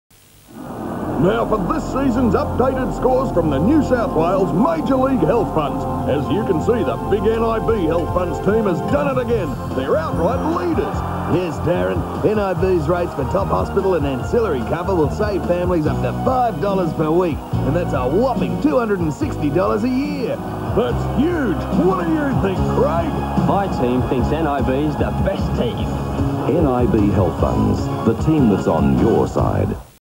NIB Health Funds - 1991 Australian TV Commercial
Recorded during a broadcast of film 'Tarzan and His Mate' at 11am on Saturday 8 June 1991 from TCN-9 Sydney.